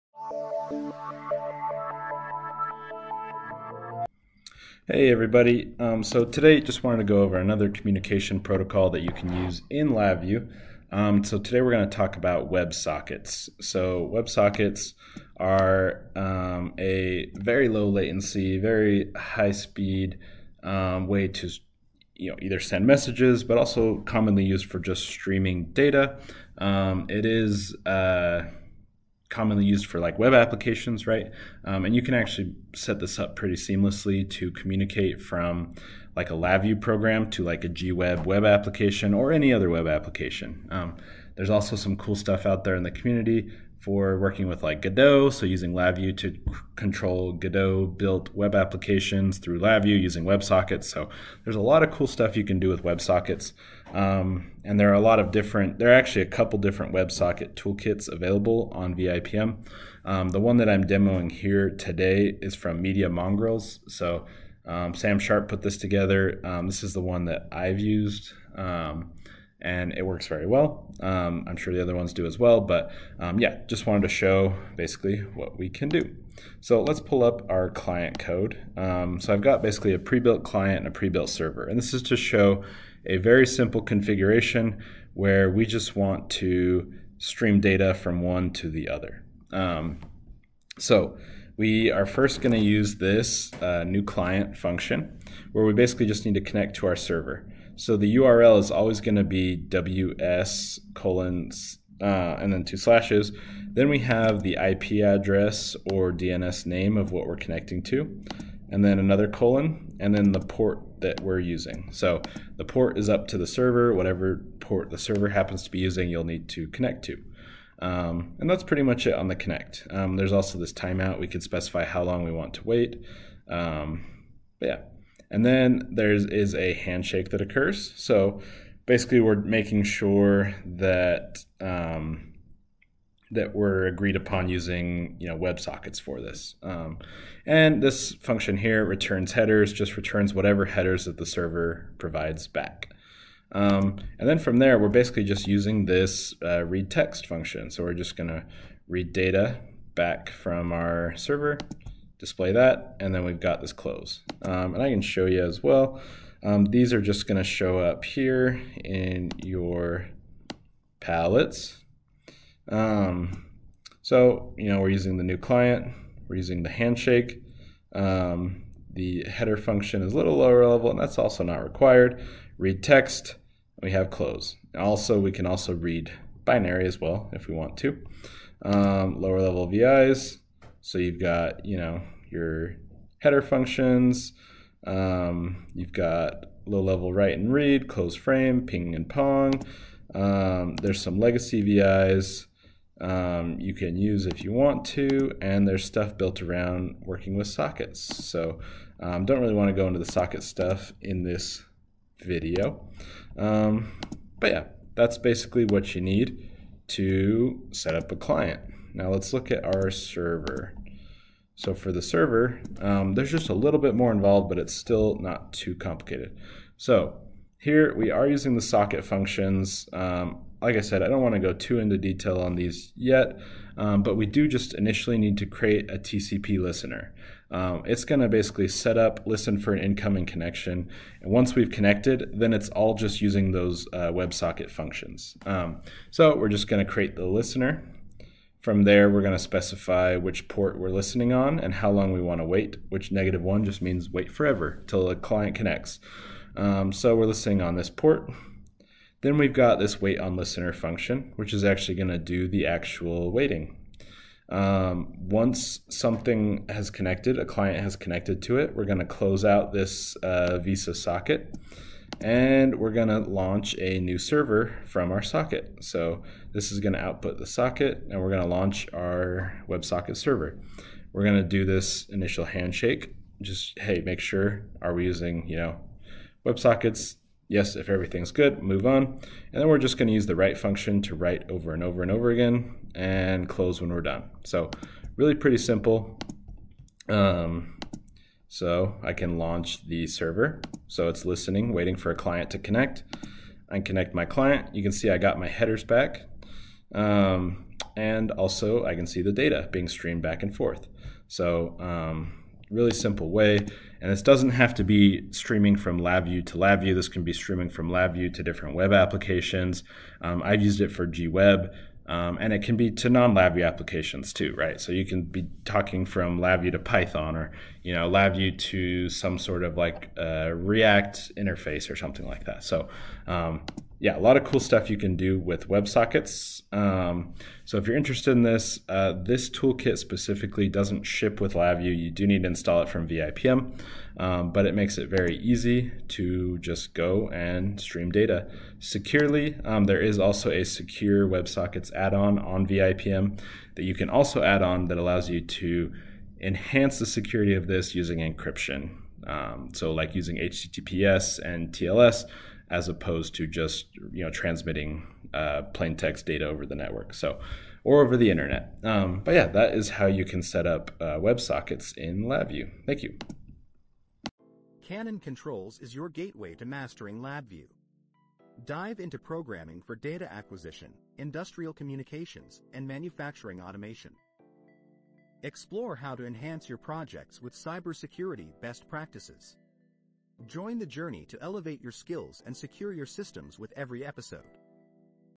Websockets in LabVIEW
Unlock the power of WebSockets in LabVIEW for real-time, bidirectional communication between your applications and web-based systems. This video takes you through the process of setting up and using WebSockets in LabVIEW, enabling seamless data exchange for modern applications.

What you’ll learn in this video:

-What Are WebSockets? An overview of how WebSockets enable real-time communication and why they’re useful in LabVIEW.
-Setting Up WebSockets in LabVIEW: Step-by-step instructions for establishing WebSocket connections.
-Sending and Receiving Data: Learn how to transmit and handle data efficiently using WebSockets.
-Practical Examples: Watch as we demonstrate WebSocket use cases, including live data streaming and web integration.
-Error Handling and Debugging: Tips for troubleshooting common issues and ensuring reliable performance.

By the end of this video, you’ll have the knowledge to integrate WebSockets into your LabVIEW projects for dynamic and interactive applications.